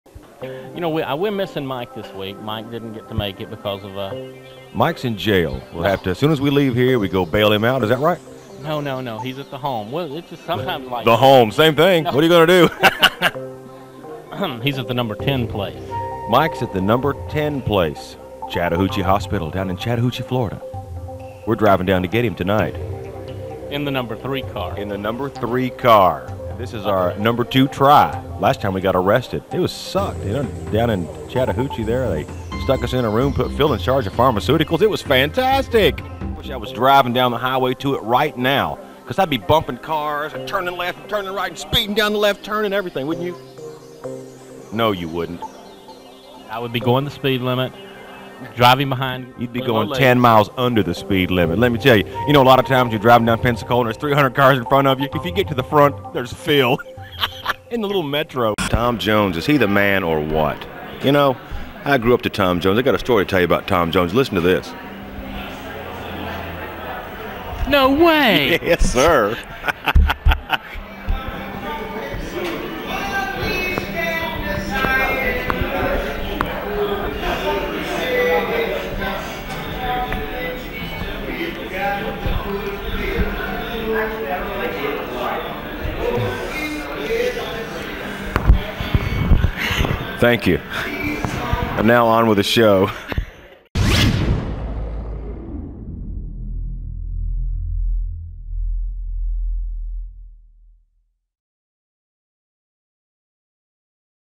The #1 Place Starring Phil & Tommy
This video stars Phil Thomas Katt & Tommy Robinetti at the #1 Place. Be prepared to laugh!